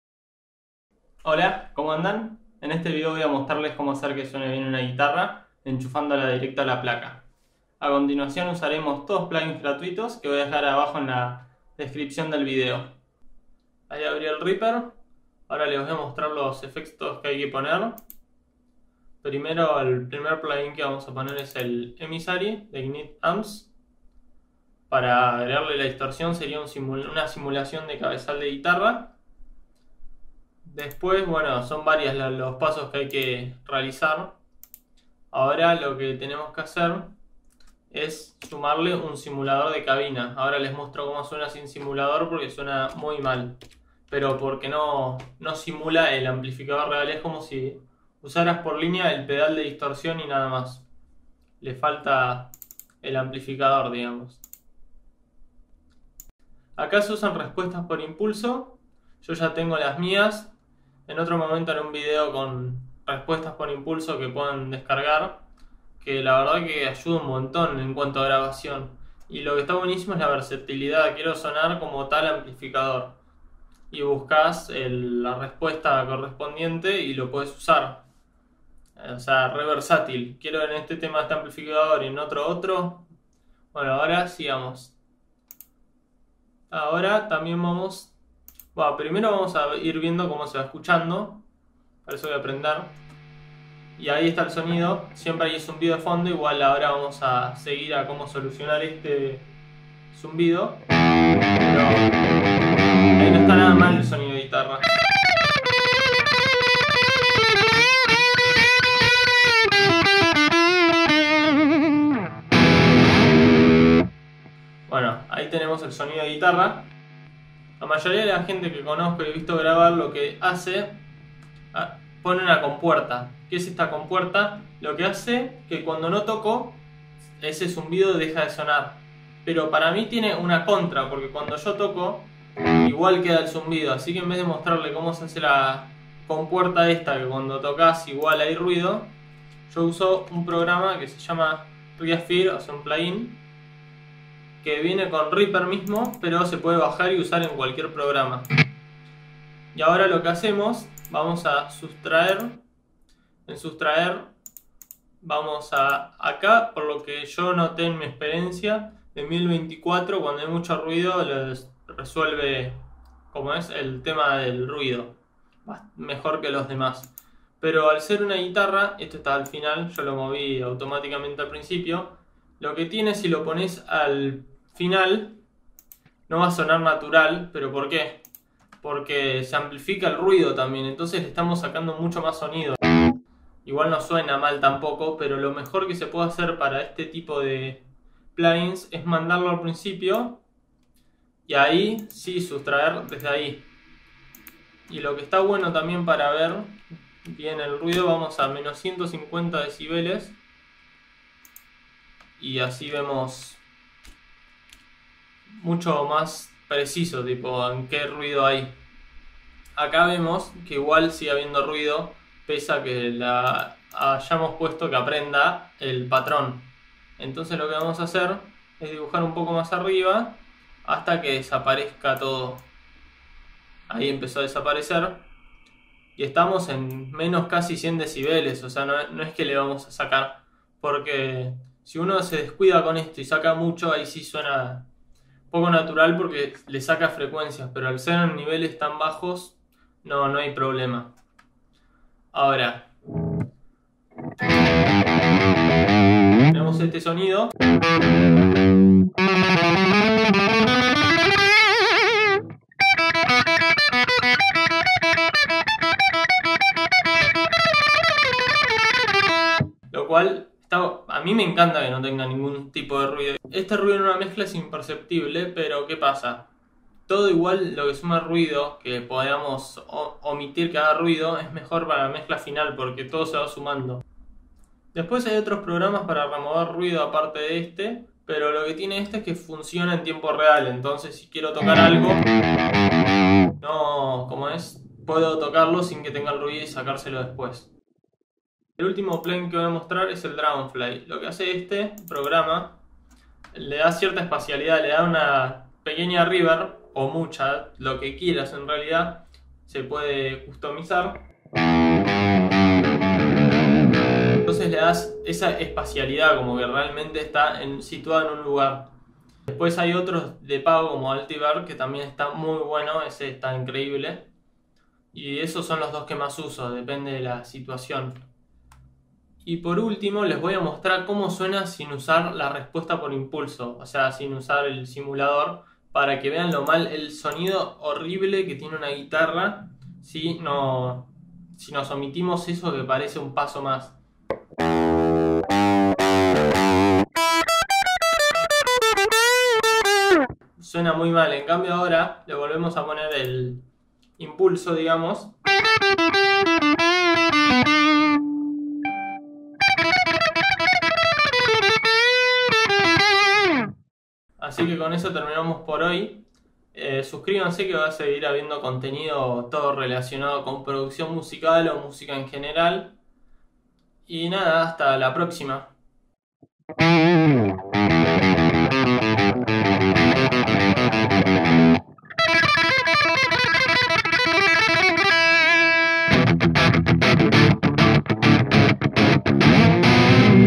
Como grabar una guitarra eléctrica usando plugins GRATUITOS!!
En este vídeo veremos como hacer sonar bien una guitarra eléctrica mediante el uso de una placa de audio y plugins gratuitos. Este método funciona en cualquier programa de grabación (DAW).

Los plugins utilizados son los siguientes:
Emissary - Ignite Amps:

Cualquier consulta escriban en los comentarios! y si hay algún interesado en masterizacion, edición de podcast o restauración de audio realizo ese trabajo.